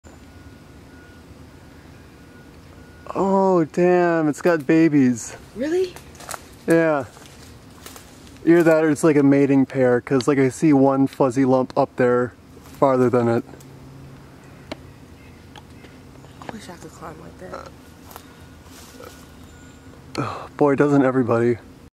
Raccoon pair climbing a tree
found these little ones while i was hiking and testing out my new canon Rebel T6i